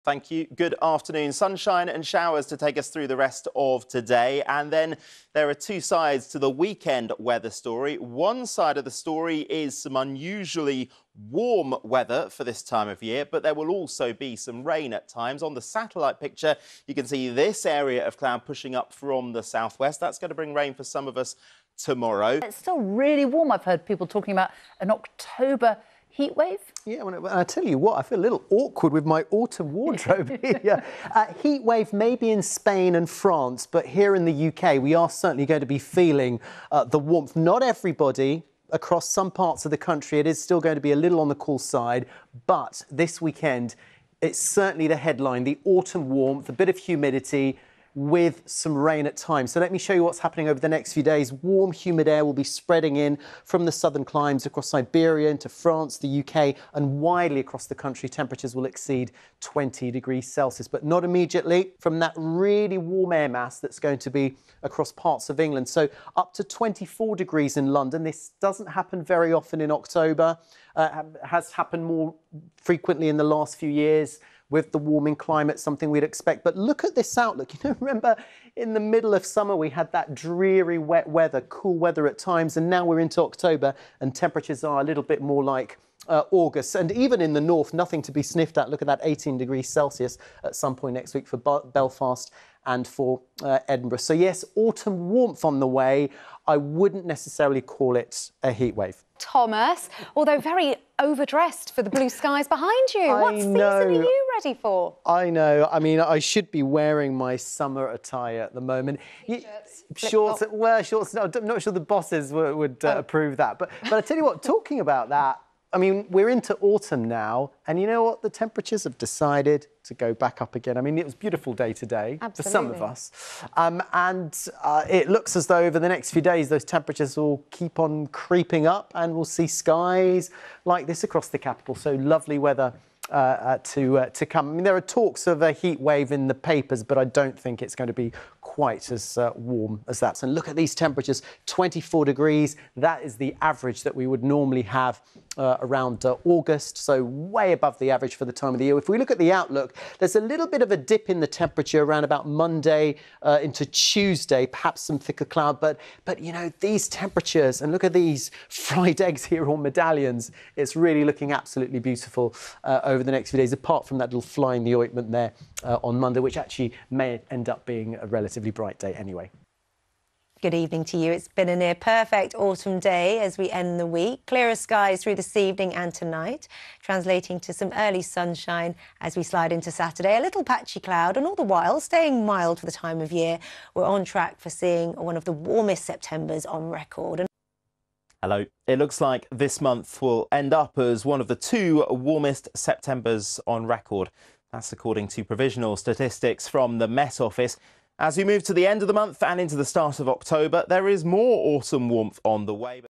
Weather images + Autumn warmth ahead (UK) BBC&ITV - 29/Sep/2023 (b)
1) Listed and unlisted (library) weather images of the day (UK).
2) Any outlooks and charts, etc. (if any).

By evening.